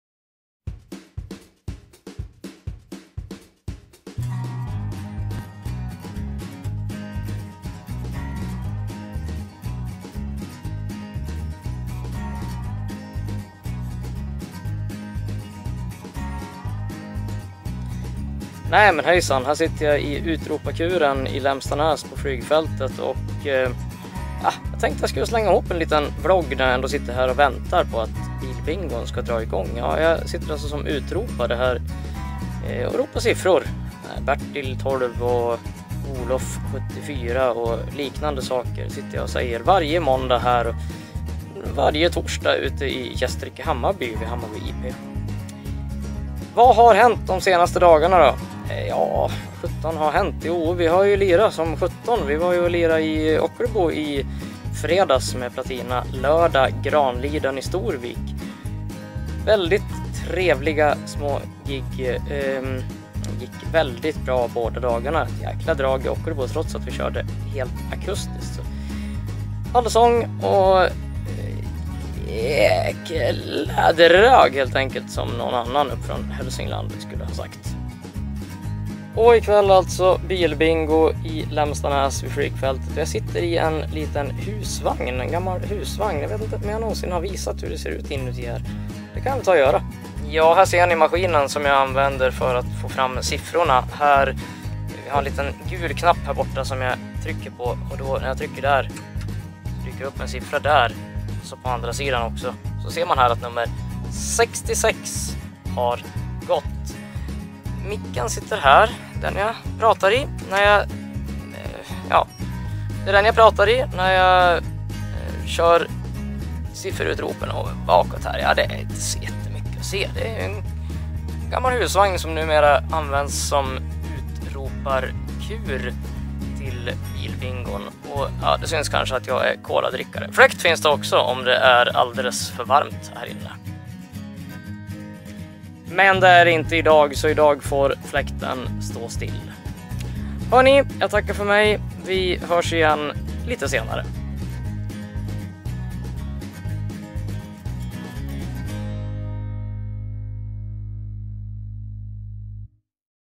PÅ INSIDAN AV EN UTROPARKUR - Johan Bakke Vlogg #48 160718
Idag vloggar jag från utroparkuren på bilbingon i Lemstanäs. Västra Gästrike Flygklubb och Torsåkers IF arrangerar bilbingo varje måndag under sommaren och de senaste åren har jag suttit som utropare på bingon, i dagens vlogg får ni se hur det ser ut där jag sitter och ropar siffror!

Jag är annars en musiker som laddar upp blandad musik, både eget och covers.